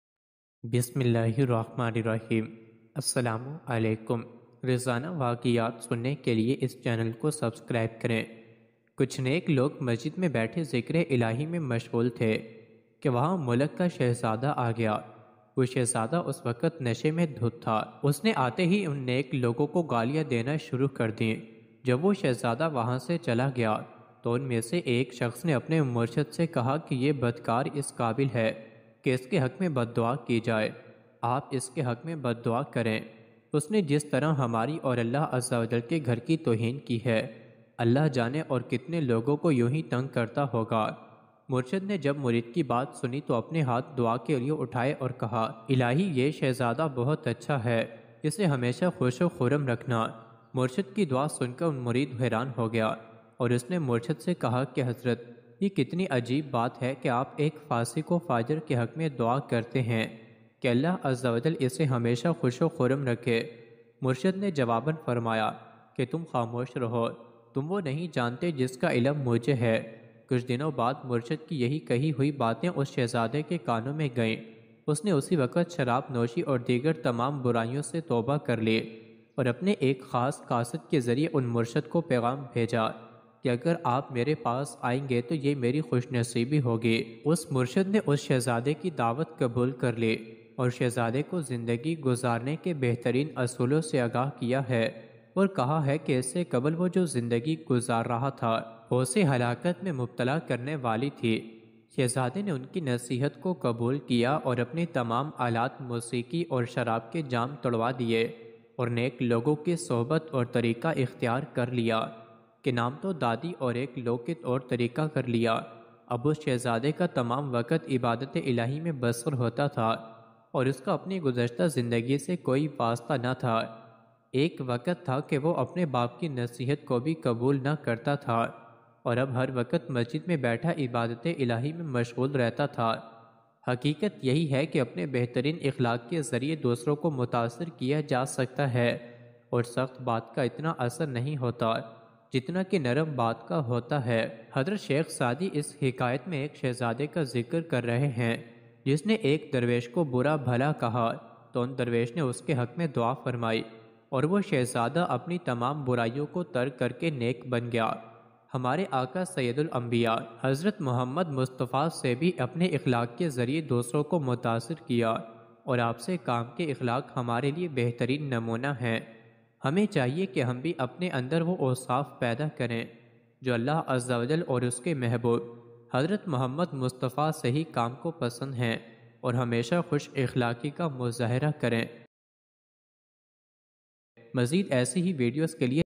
Story Of Pious People | Naik Logo Ka Waqia | Moral Islamic Story
Some pious people were sitting in the mosque engaged in divine remembrance when the prince of the country came there. That prince was drunk at that time, he started abusing these good people as soon as he came. When that prince left there, one of them said to his mentor that this evil character deserves to be prayed for, how many people will be harassed. When the Murshid heard the murid's words, he raised his hands to pray, then his senses were blown away This...?